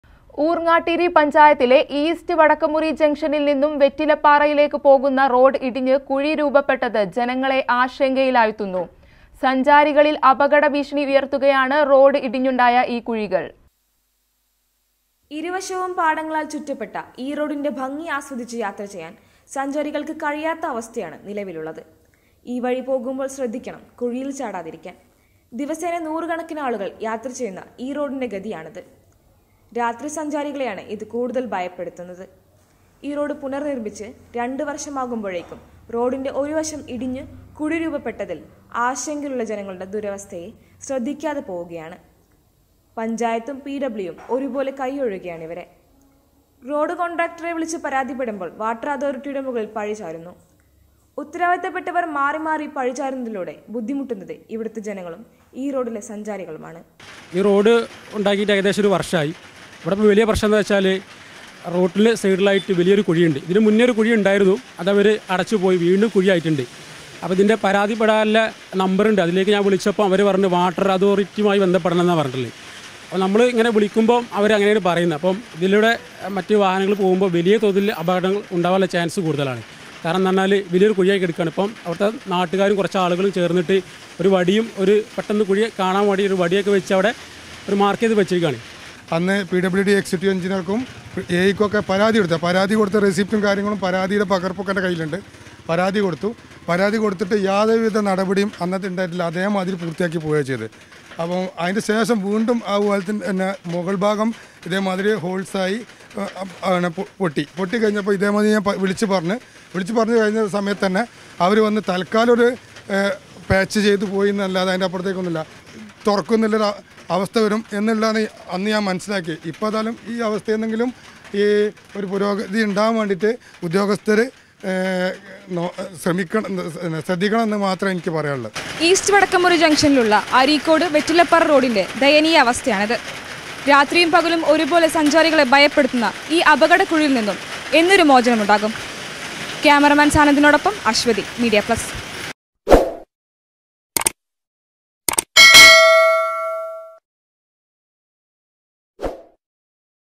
അപകട കെണിയില്‍ വെറ്റിലപ്പാറ ജനങ്ങള്‍...| MEDIA PLUS TV
അപകട കെണിയില്‍ വെറ്റിലപ്പാറ ജനങ്ങള്‍.റോഡ് ഇടിഞ്ഞ് കുഴികള്‍ രൂപപ്പെട്ടിട്ടും നാട്ടുകാരുടെ പരാതിയില്‍ ഇടപെടാതെ അധികൃതര്‍...| MEDIA PLUS TV |